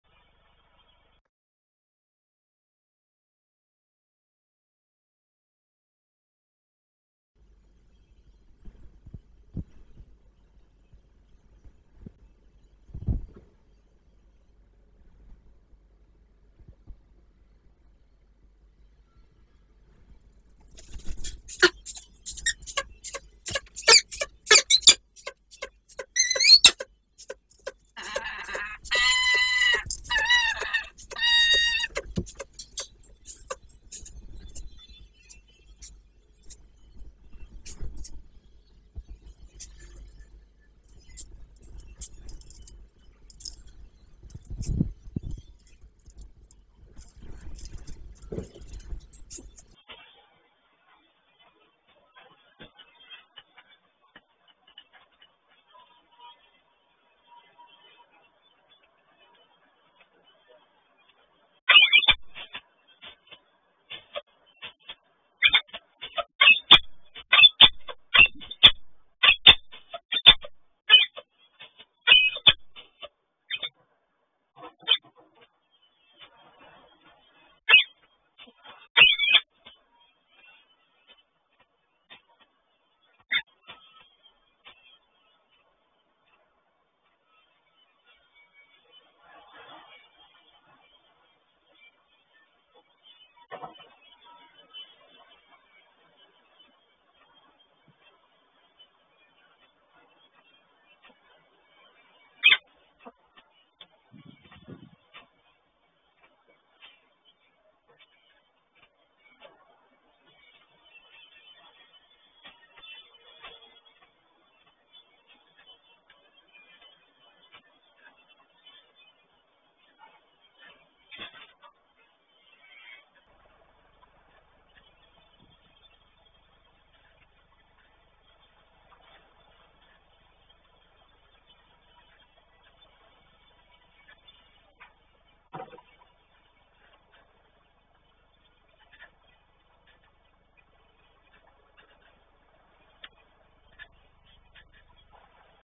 PGNiG Termika Kawęczyn PL- Apasz on shift , Czarnooka arrived , He on landing - 2024 04 03 09 18
They talked, he flew